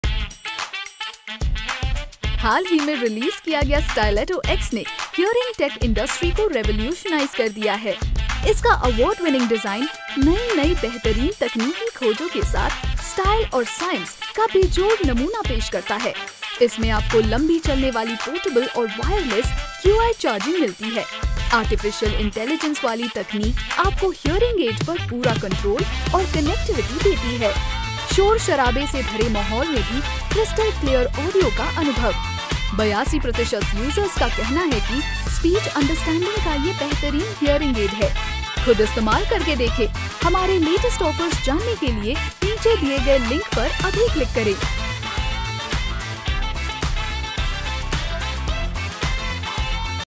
देखें 2024 के लेटेस्ट हियरिंग एड्स कैसे दिखतें और क्या कर सकते हैं ~vb3208
आप अभी जांच सकते हैं कि क्या आप इन जर्मन श्रवण यंत्रों का परीक्षण करने के योग्य हैं।

क्या आप:
- 50 वर्ष या उससे अधिक
-सुनने में दिक्कत होना
- भारत में रहना